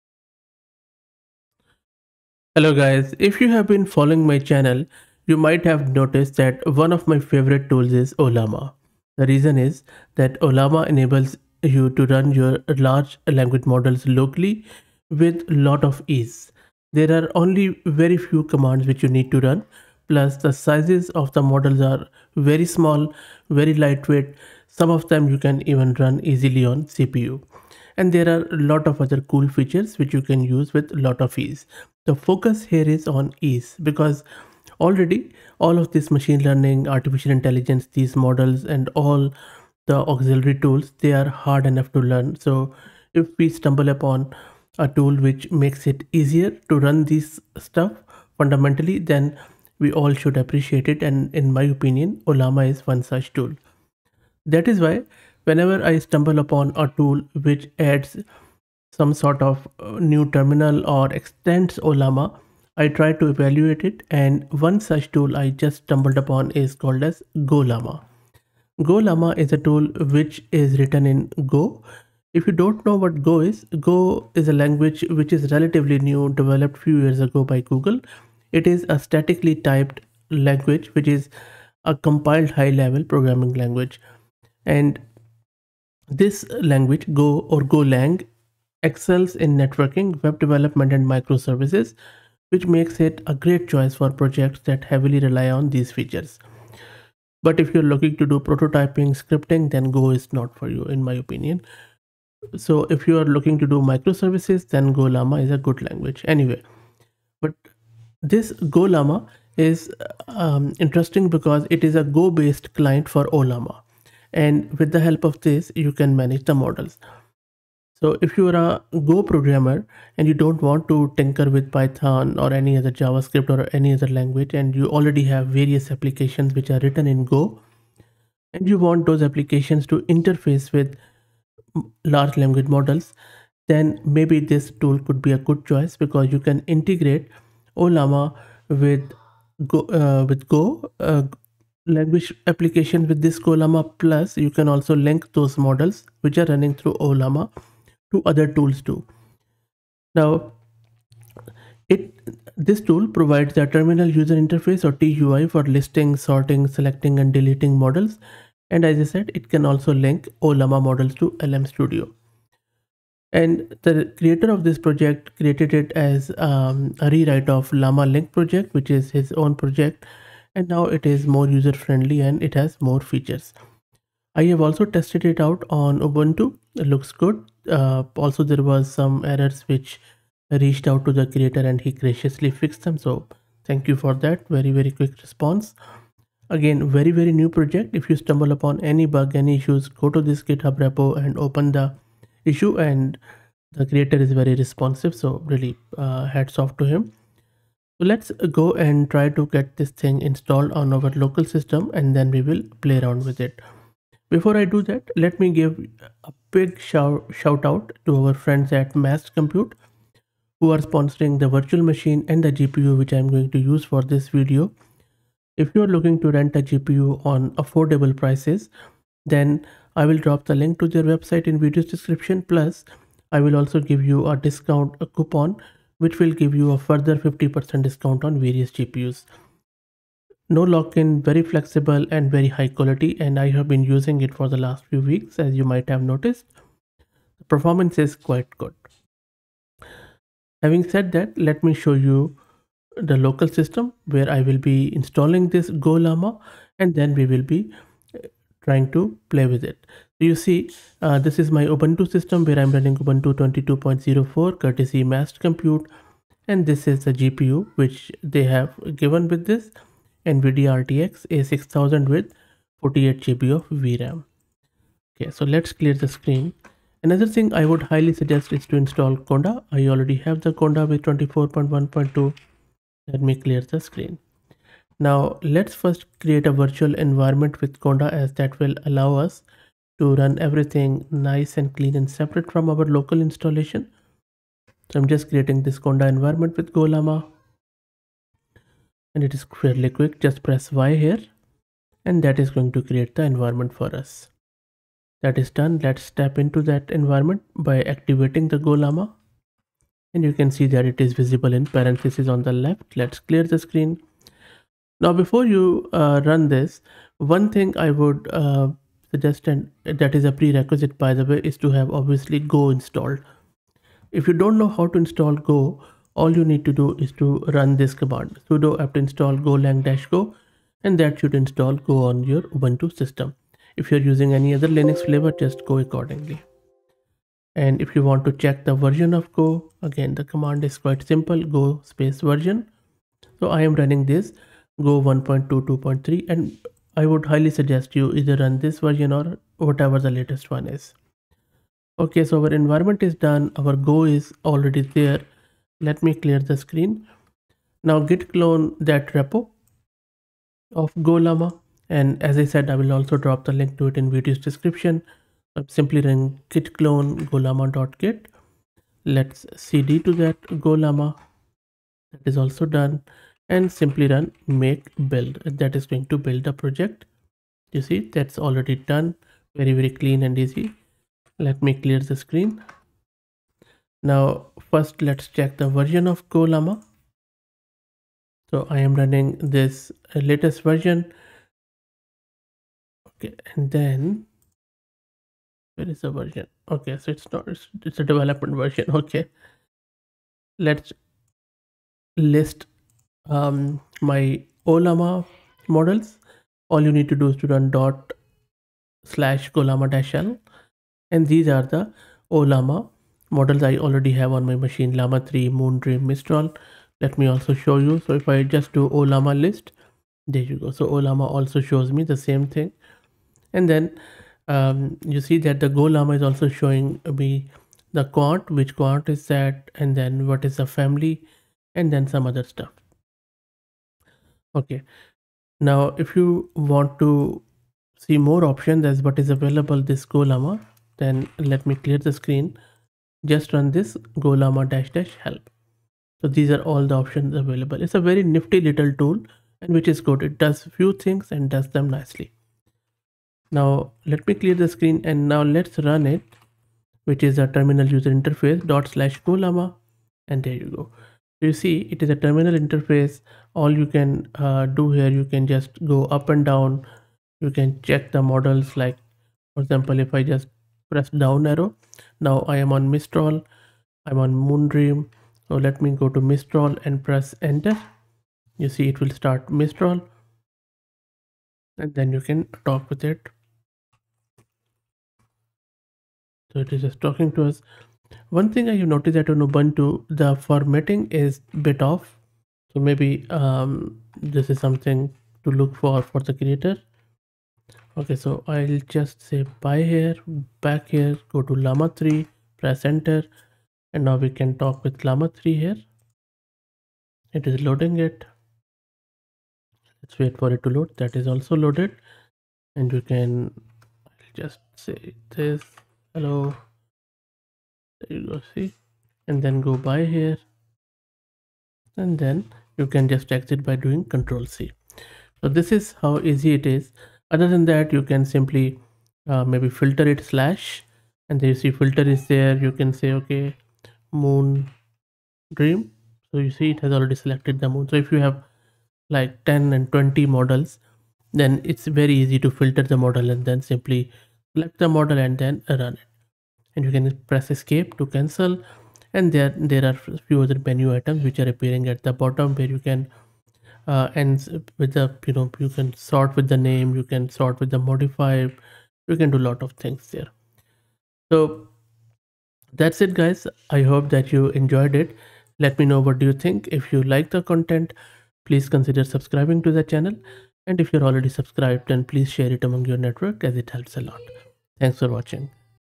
Gollama - Manage Ollama Models Locally with Go
This video installs Gollama locally which is a Go-based client for Ollama for managing models. It provides a TUI for listing, sorting, selecting and deleting models and can link Ollama models to LM-Studio.